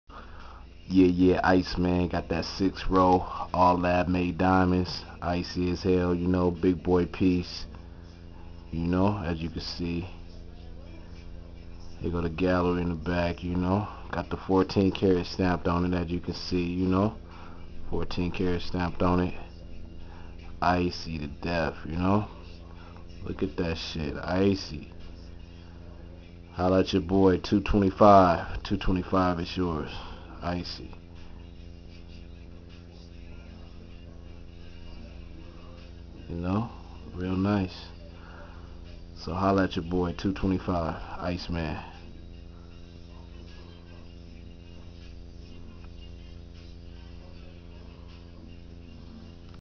LAB MADE DIAMOND 6 ROW BRACLET ICEY AS HELL $225.00
LAB MADE DIAMOND 6 ROW BRACLET BIG VVS1 IN CLARITY SLIVER DIP IN PLATNIUM NEVER FADE HIGH QUALITY JEWELRY STAMPED 14KT ON IT ASK FOR ICEMAN AT OR HM.971-255-0663 I ACCEPT PAYPAL OR WESTERN UNION OVERNIGHT SHIPPING WONT LAST LONG SO HOLLA.